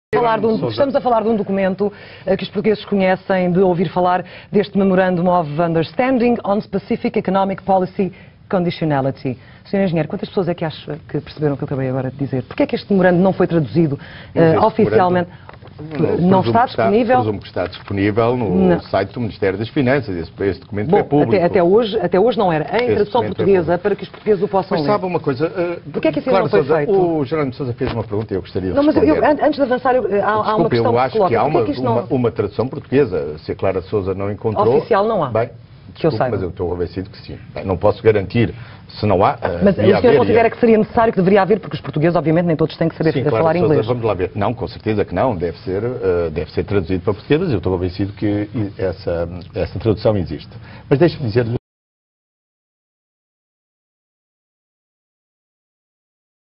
A tradução do memorando existe
Perguntado sobre o memorando, e porque não existe uma tradução oficial do acordo com a troika, Sócrates está convencido que existe, e existe, em trabalho de grupo e aberto no Aventar.